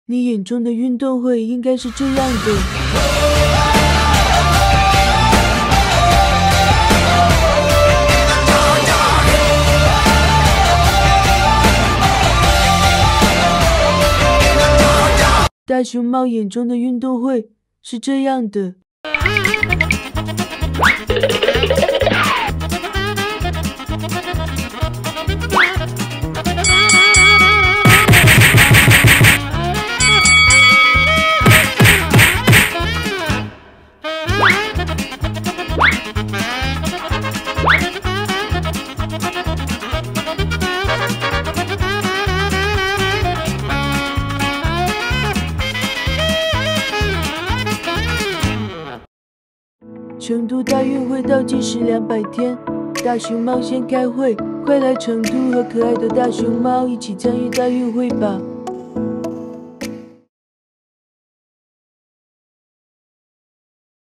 Have You Seen the Gaint Pandas Held “Sports Games”？大熊猫“运动会”你见过吗？
What do you think about the sports games?
Are you interested in the “gaint pandas games”?
“Chengdu 2021 FISU WUG” will be open in 200 days, the “gaint pandas games” has been held as a warm-up.
Gaint panda Wrestling, Pole Vault, Swiming，skiing.
Let’s see these cute giant pandas!
你眼中的运动会是什么样的？大熊猫“运动会”你见过吗？今天，距离“成都2021年第31届世界大学生夏季运动会”正式开幕仅剩200天啦！可爱的大熊猫们特意准备了一场“运动会”！大熊猫摔跤、撑杆跳、游泳、滑雪. 赛事激烈，一起来看看吧！